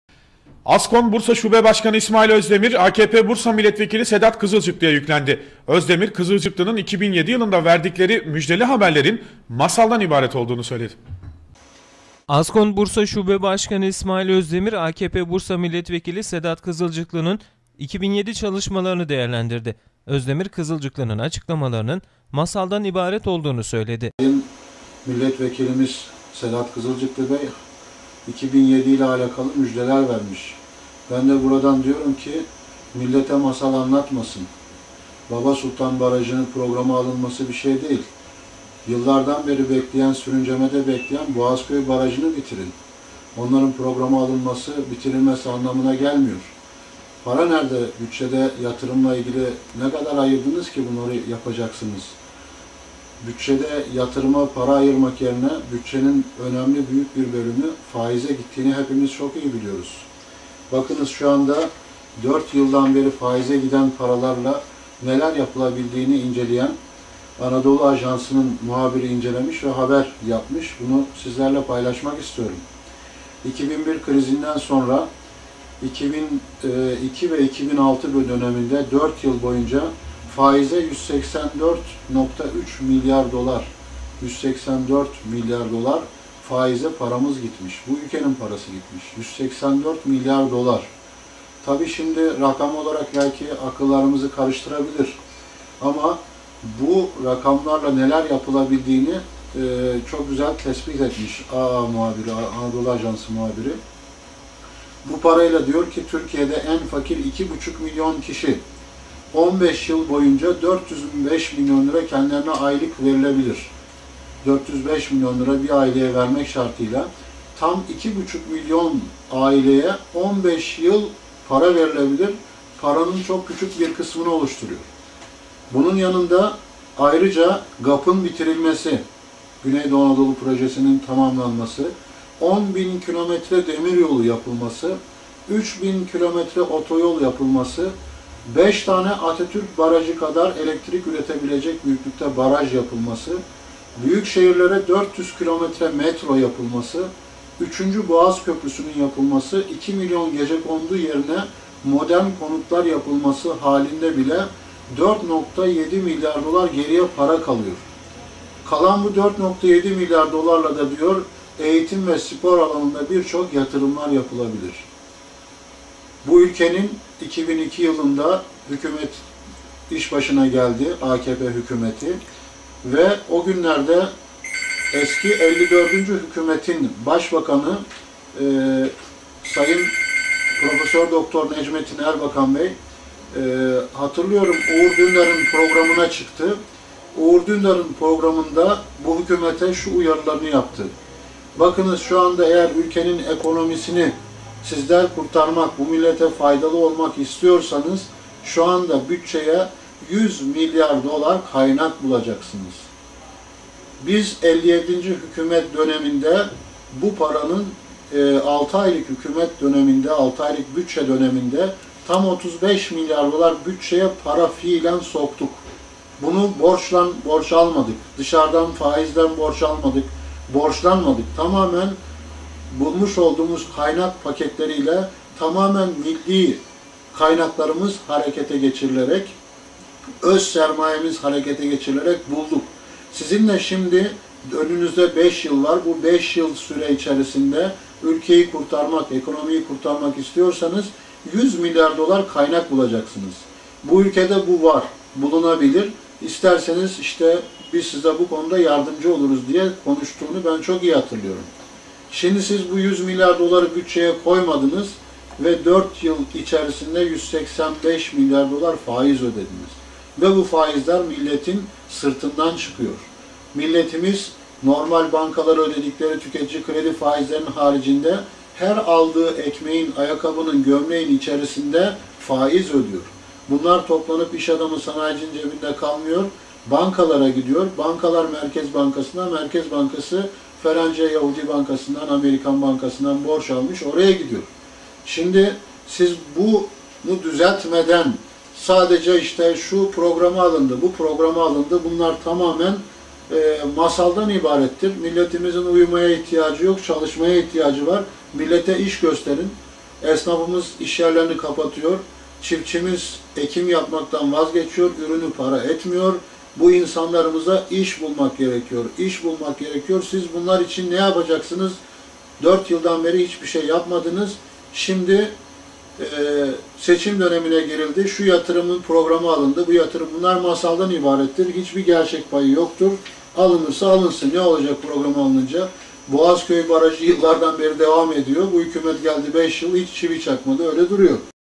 İSMAİL ÖZDEMİR'DEN SEDAT KIZILCIKLI'YA ELEŞTİRİ:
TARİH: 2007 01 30
YER: İNEGÖL
KONUŞMACI: İSMAİL ÖZDEMİR (ASKON BURSA ŞUBE BAŞKANI)